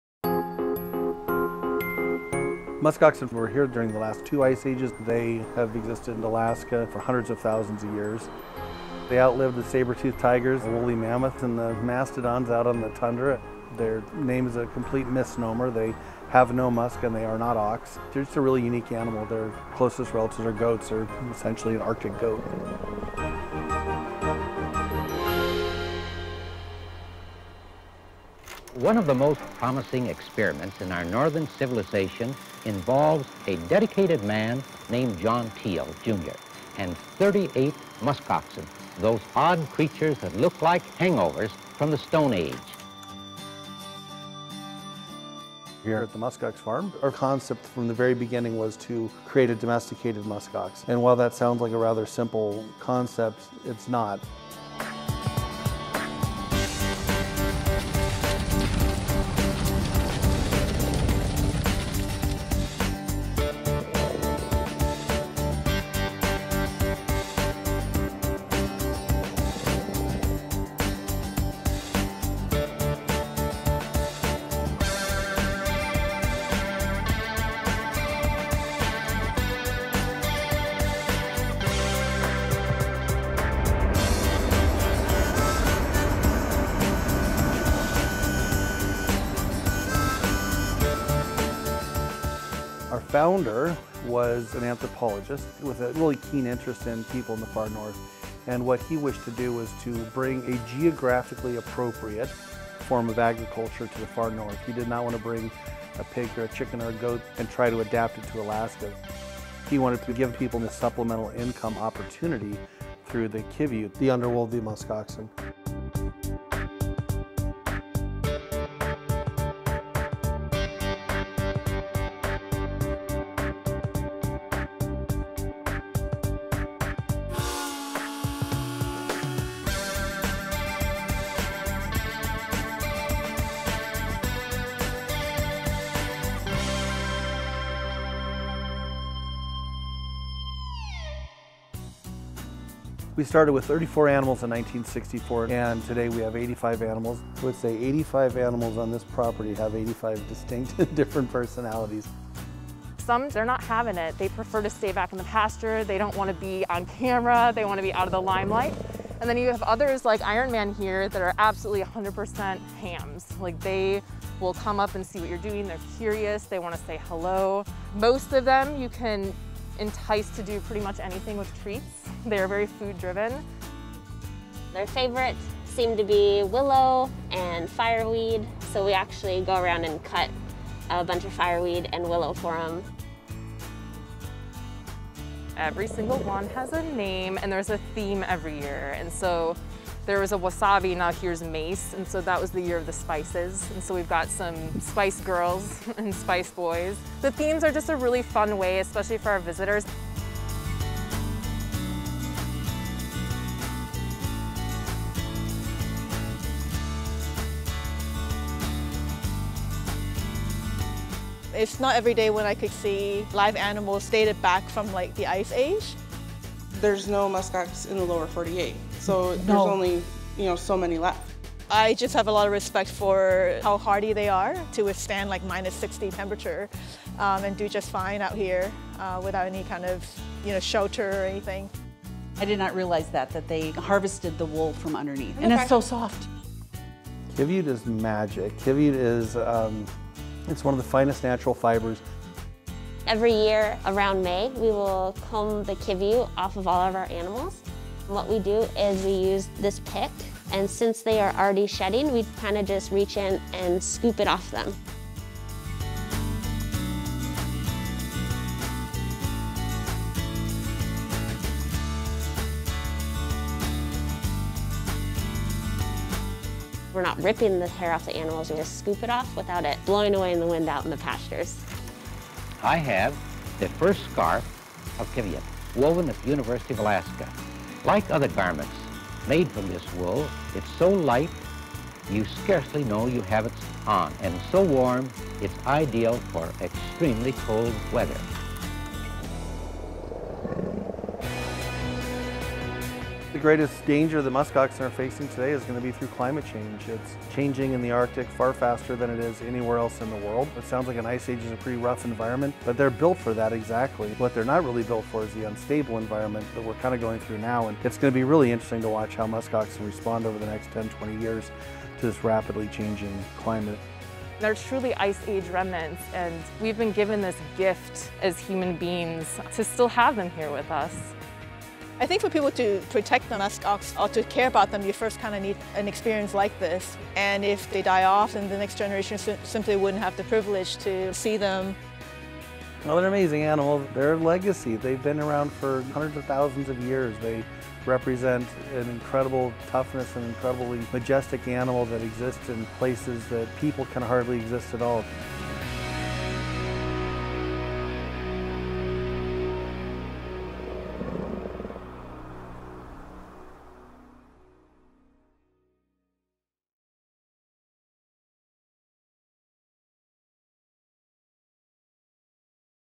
Musk Oxen Have Survived Two Ice Ages; Can They Survive Climate Change? | NBCLX
The musk ox outlived the saber-toothed tigers, woolly mammoths and mastodons. But despite surviving two ice ages and coming back from near extinction, their greatest threat to date might come from the unstable temperatures of climate change. Today, a musk ox farm in Alaska is working to preserve these ice age throwbacks for future generations by domesticating them.

Directed & Produced by: Matt Dworzańczyk
Production by: Etherium Sky Media
Archival Footage Courtesy of: Alaska Film Archives - University of Alaska Fairbanks; Musk Ox Farm